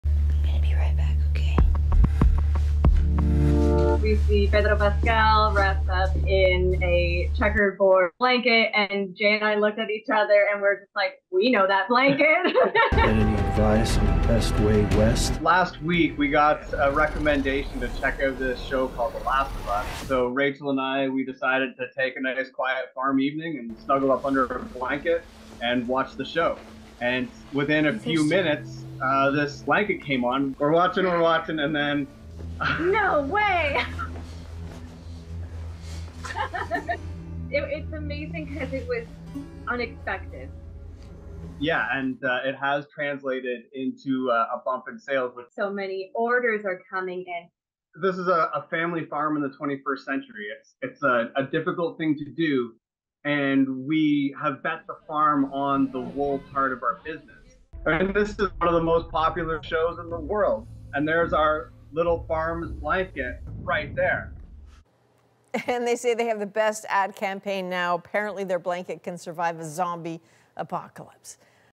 TheMoment wool farmers spot their blanket on The Last of Us 🧟‍♂️
A green blanket made by a small wool farm in Ontario made a cameo on HBO’s hit show The Last of Us, leading to big business for the farm.